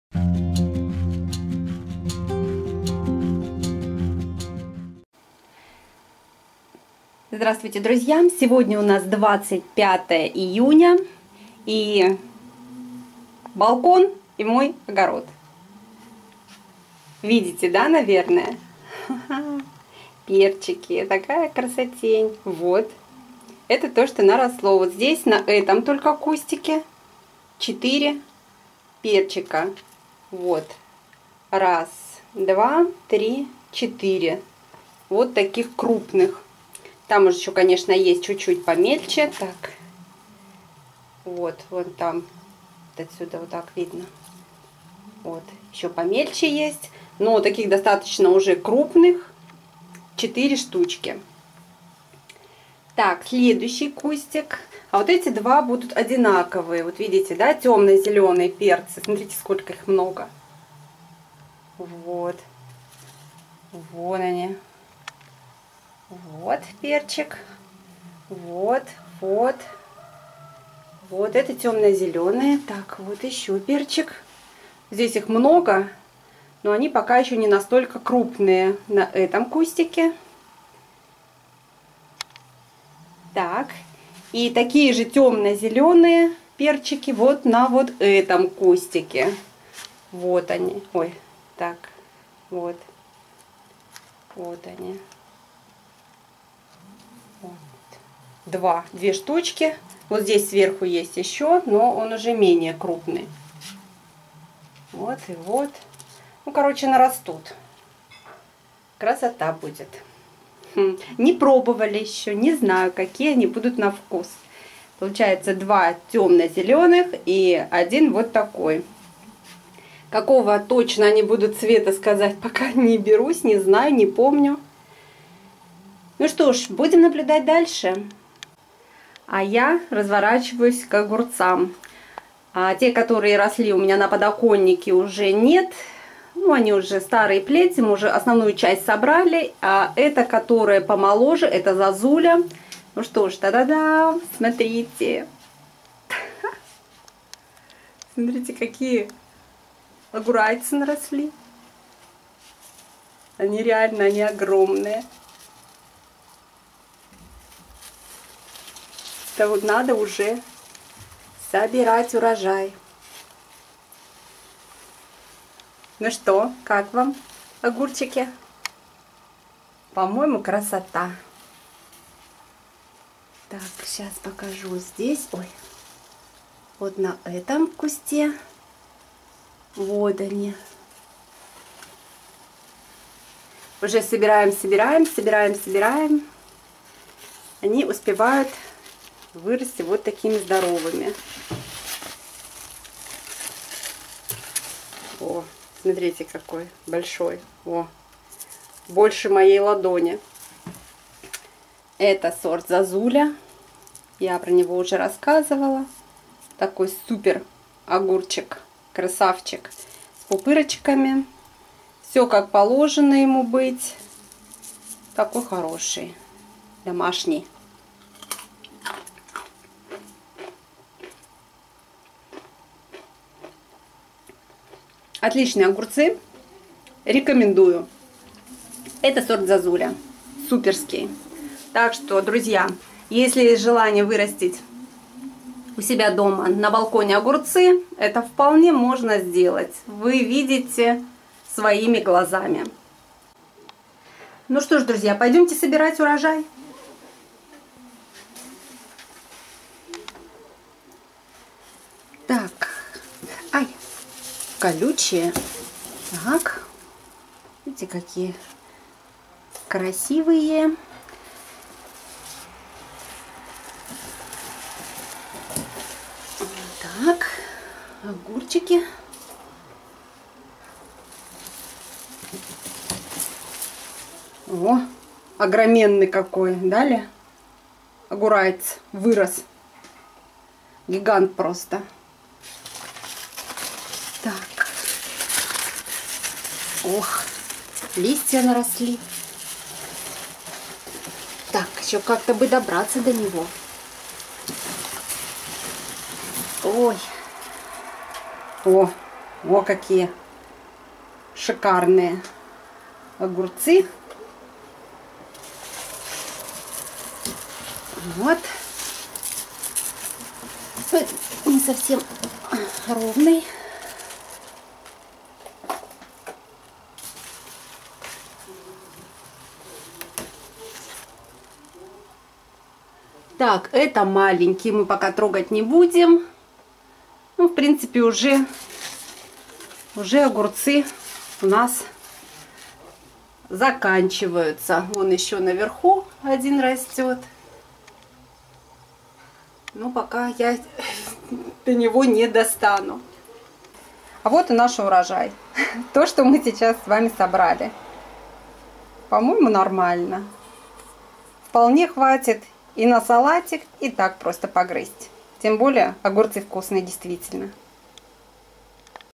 Супер-огурцы! Огород на балконе 25.06.2022
Собираем урожай огурцов на балконе. Огород на балконе.
Посмотрите, как выросли на балконе мои перцы. Я очень довольна.
Выращиваю дома